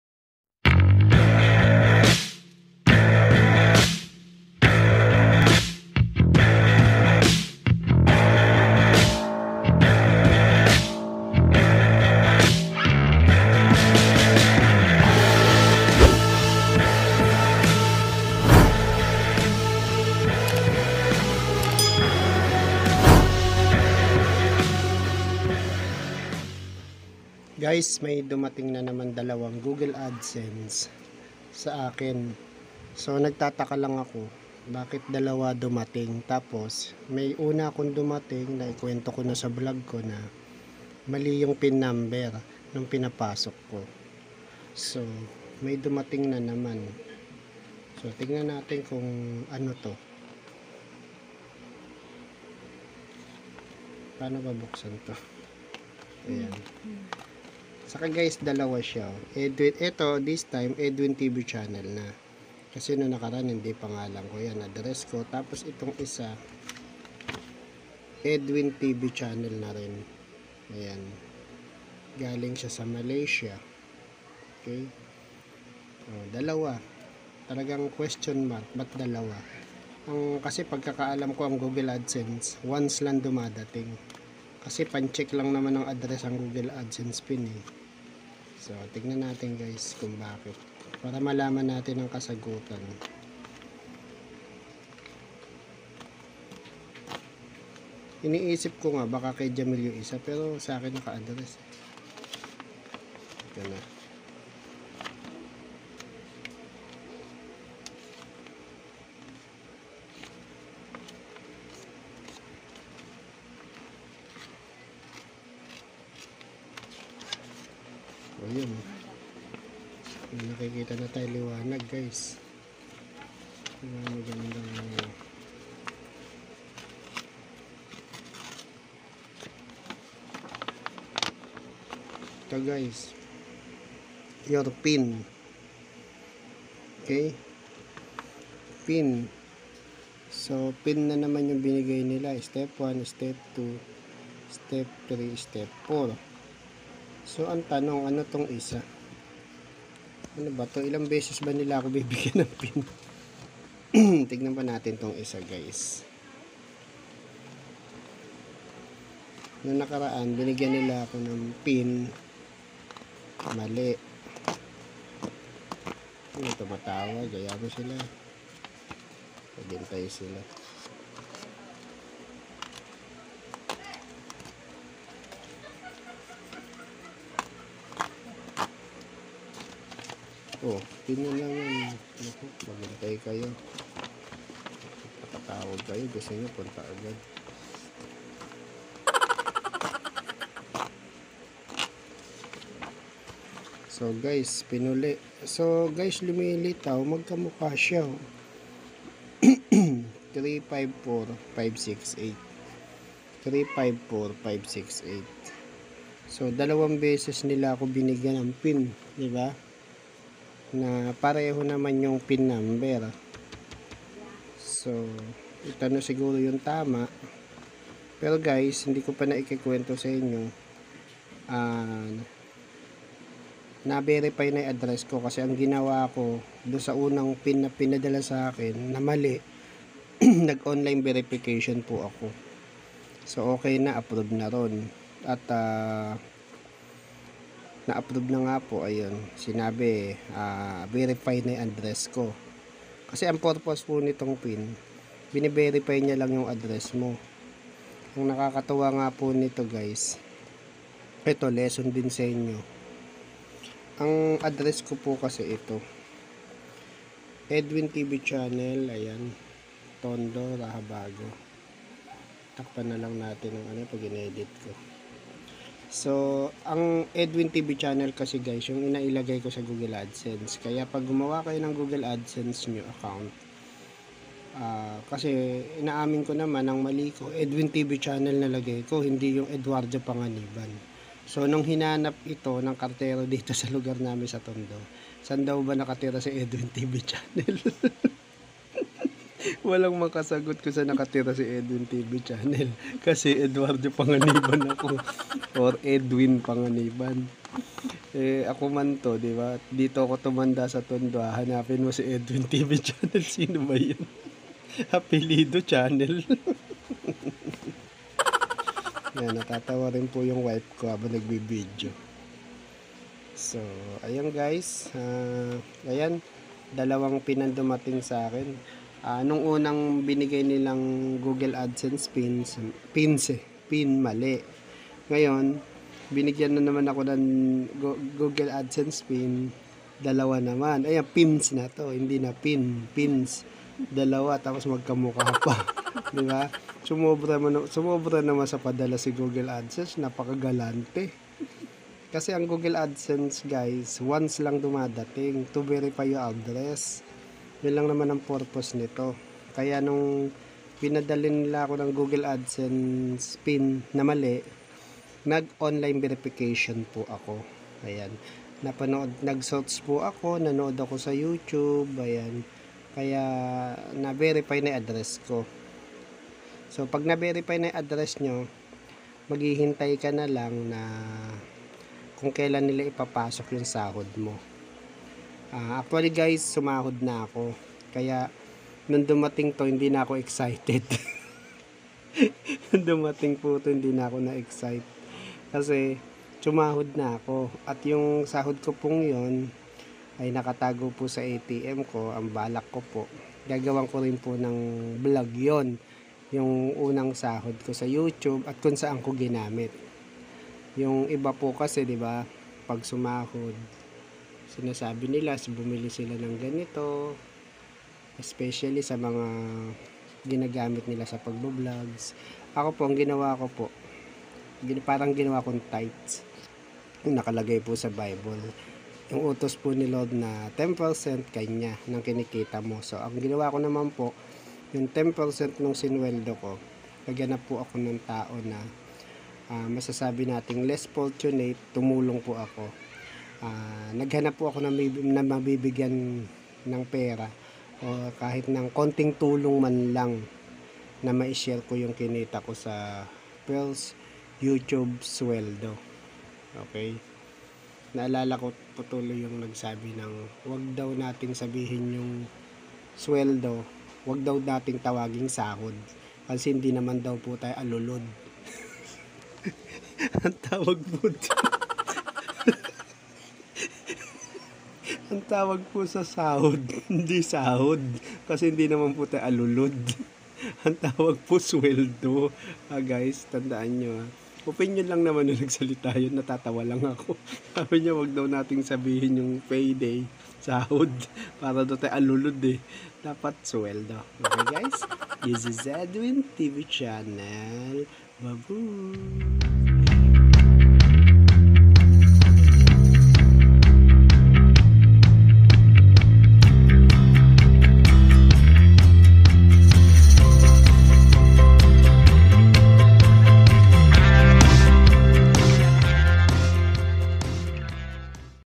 Two (2) Google Adsense PIN Delivered in my house plus the first Google adsense PIN. OMG 😳
Ang dami naman pinadala ni Google adsense ng PIN sa akin, yun iba naghihintay pa pero ako bale  tatlo pa pinadala .